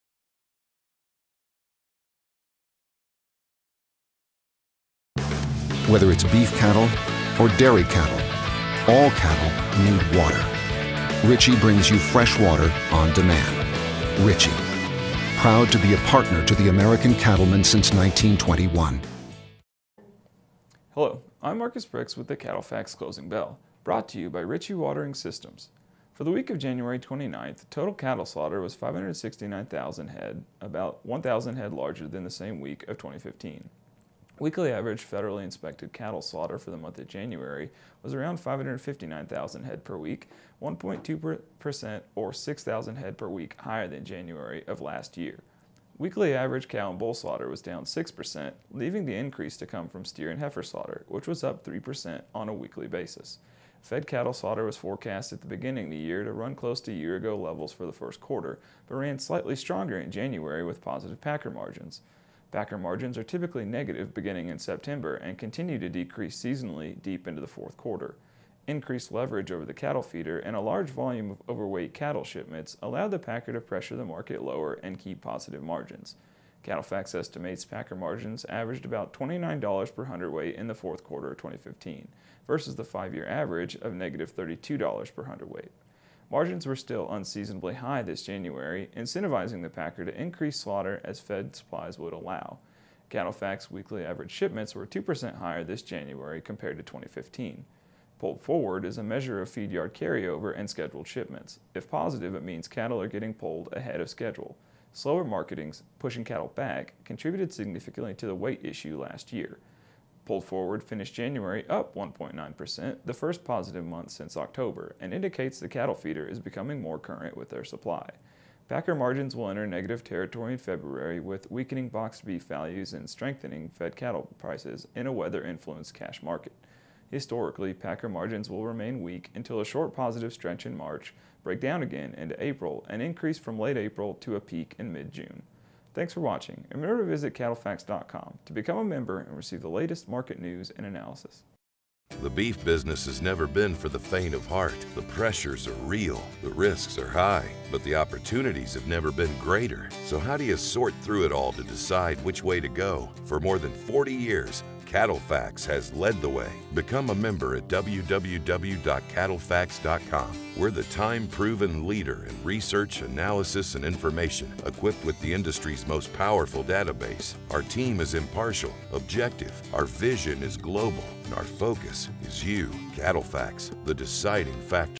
Beef Packing Margins Support Slaughter
CattleFax analyst Marcus Brix discusses seasonally strong margins for beef packers and how this influences cattle slaughter volumes in January.